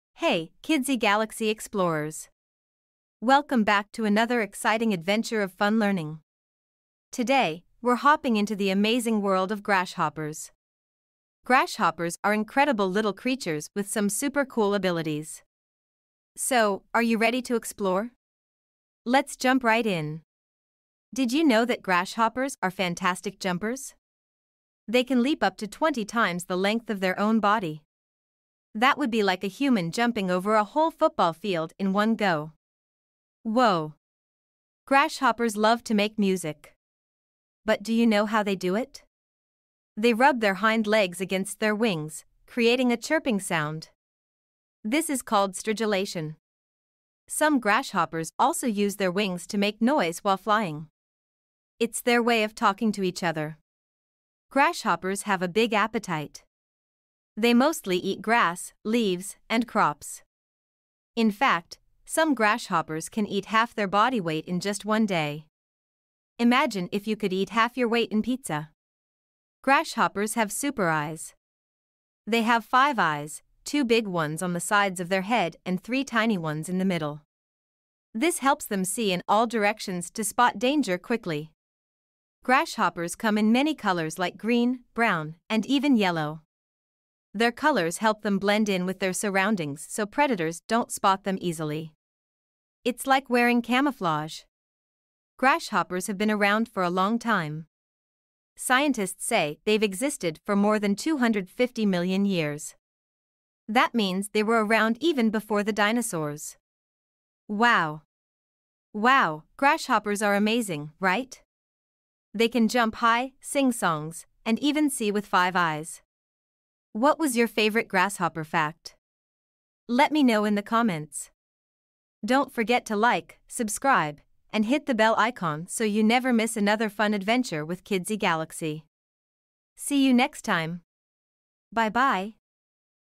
🦗 Amazing Grasshopper Facts for Kids! | Facts about Grasshopper
🌟 Welcome to Kidzy Galaxy, where learning is always fun and exciting! 🚀 In today’s video, we’re hopping into the world of grasshoppers and discovering some mind-blowing facts about these amazing little creatures! 🦗✨
📌 In this video, you’ll learn:
✅ How high grasshoppers can jump! 🏃💨
✅ How they make music with their legs! 🎵
✅ What they love to eat! 🍃
✅ Why they have FIVE eyes! 👀
Grasshoppers have been around for over 250 million years—even before dinosaurs! 🦖 So, get ready for a fun adventure filled with amazing insect facts!
🔔 Don’t forget to:
✅ LIKE 👍 this video if you love learning new things!